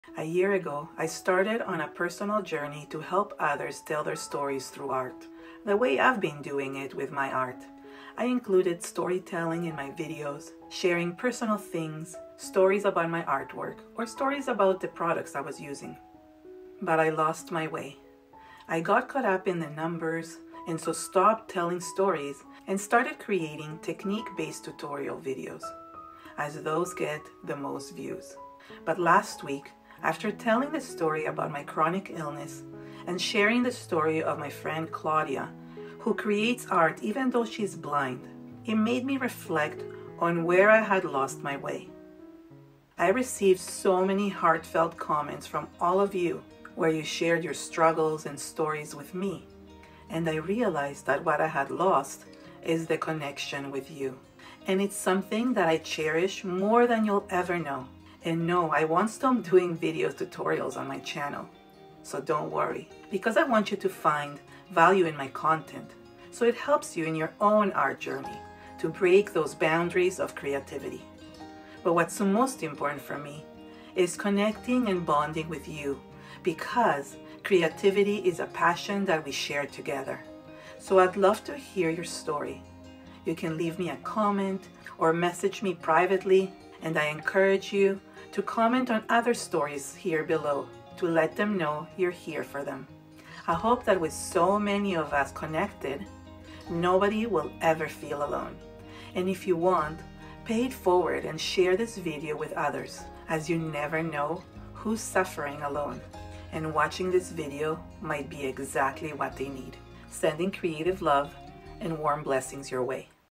I LOST MY WAY 😢 Why Did This Happen?
Sometimes we veer off our path, getting distracted with things that in the long run, don't fulfill us as they are just numbers. I lost my way. but I'm finding my way back!

This is how I earn compensation for all the hard work I do. The information provided in this video is based on my own personal experience You can read my full disclaimer in the About Me section of my